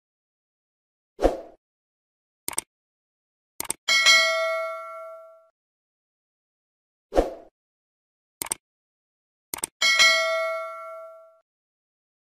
Bell Effect Subscribe Overlay for Watermarks || NO COPYRIGHTS
Reduce the size of this overlay to use as bell effect for your channel watermarks!   Then drag it above your channel watermark.  See my latest videos for the sample!
Please feel free to use.   NO COPYRIGHTS!